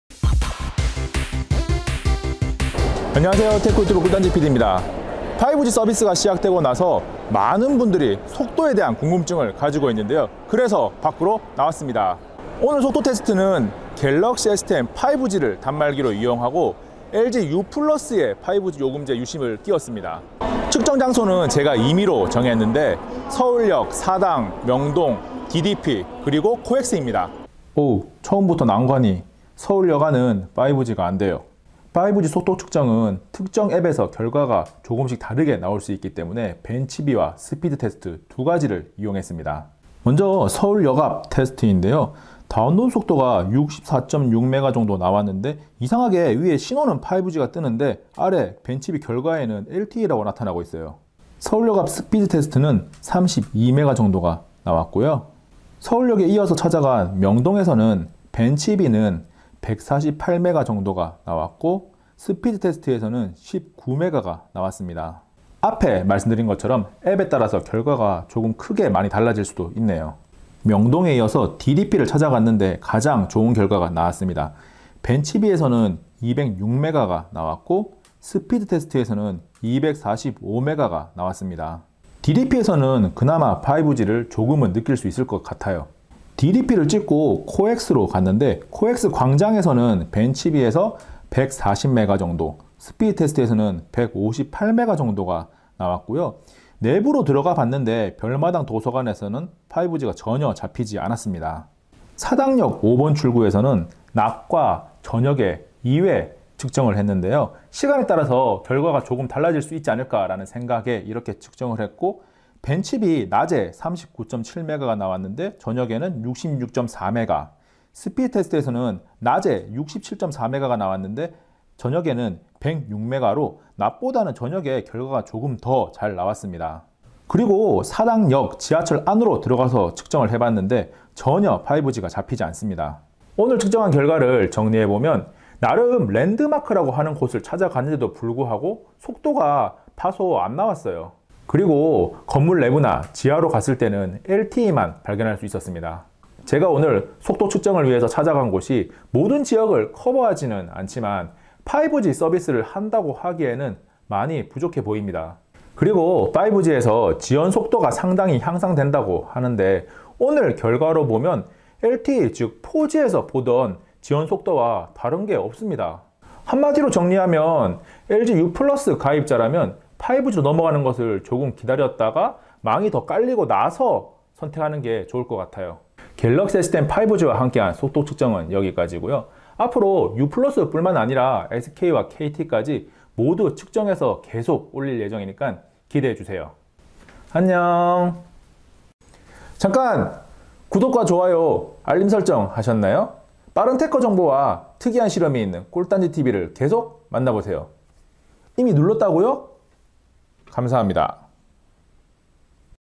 [5G 속도측정 1편] 갤럭시S10 5G X LG유플러스! 5G 맞아? (서울역, 사당, 명동, DDP, 코엑스)
4월 3일 23시 이동통신 3사의 5G 서비스가 시작되었고, 4월 5일부터는 일반 가입도 시작되었습니다. 그래서 갤럭시S10 5G에 LG유플러스 5G 요금제 유심을 장착하고 속도 테스트를 나갔습니다. 테스트 장소는 서울역, 사당, 명동, DDP, 코엑스 입니다. 앞으로 계속해서 이동통신 3사 모두의 속도측정 결과를 계속해서 올릴 예정입니다. 이번 테스트 결과는 영상을 통해 확인해주세요! 다소 실망할지도 모르겠네요!

구독을 누르면 꿀단지TV 영상으로 빠르게 만나볼 수 있어요!